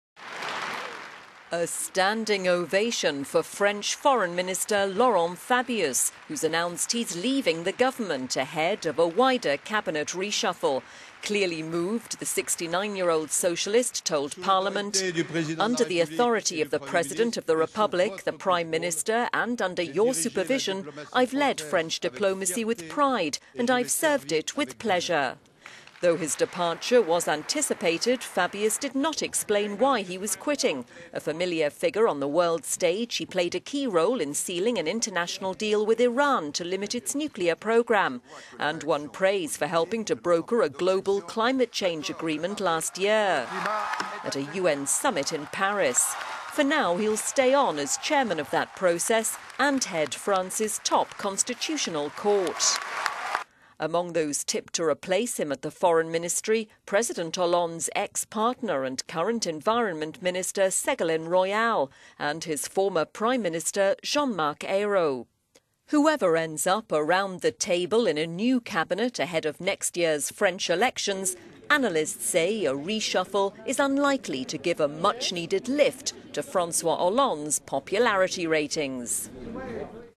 French Foreign Minister Laurent Fabius bows out of government
French Foreign Minister Laurent Fabius received a standing ovation in the country's National Assembly on Wednesday after announcing that he was leaving the government, ahead of a wider cabinet reshuffle.

Clearly moved, the 69-year-old Socialist told MPs:

"Under the authority of the President of the Republic, the Prime Minister and under your supervision, I have led French diplomacy with pride and I have served it with pleasure."

Although his departure was widely anticipated, Fabius, who was …